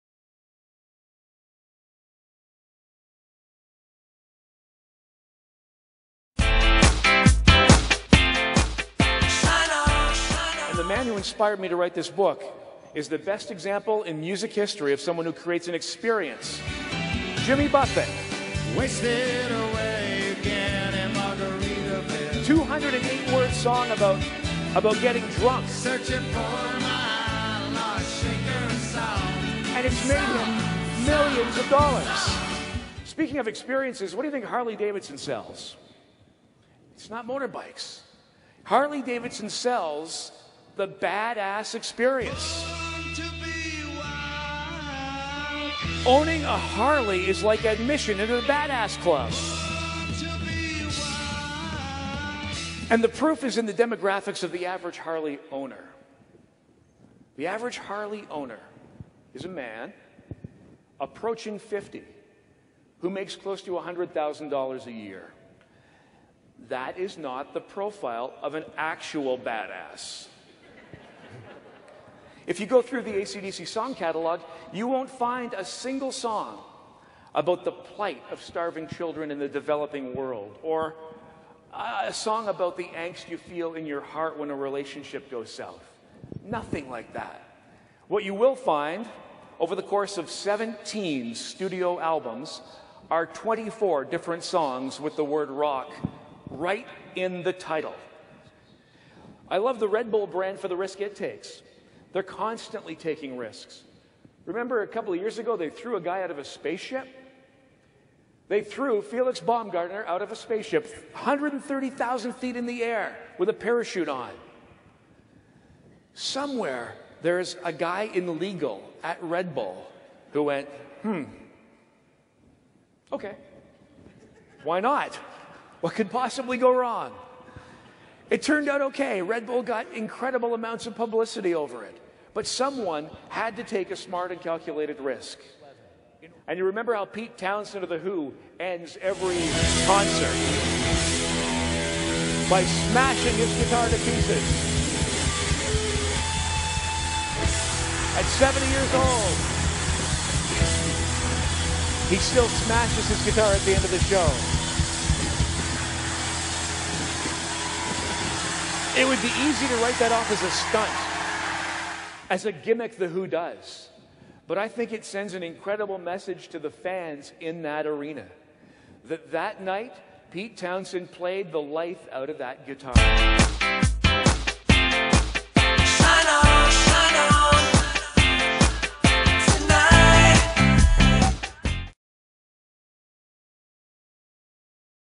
Steve Jones - Keynote Speaker
Steve Jones is a 30-year music industry insider, one of the world’s leading radio programmers, and author of two highly-acclaimed business and marketing books, Brand Like a Rock Star and Start You Up. Steve taps into his experiences in the music biz to illustrate what great businesses can learn from the legends of rock.

Gene Simmons of Kiss says, “Steve Jones knows a thing or two. Listen and take notes.”

In engaging concert-like presentations filled with backstage stories, music, and videos, Steve shows how great brands can be as consistent as AC/DC, overcome adversity with the resilience of Fleetwood Mac, build incredible fan experiences like Jimmy Buffett, and connect with customers like Taylor Swift.   Notorious rock star Alice Cooper says, “Steve gives you the insight of a rock ‘n roll veteran.”

Steve has created and guided winning media brands in the US, Canada, and the Caribbean.  He was recognized as International Radio Programmer of the Year at the 2014 Worldwide Radio Summit, and currently oversees 90+ radio brands in Canada as VP/Brands & Content with Newcap Radio.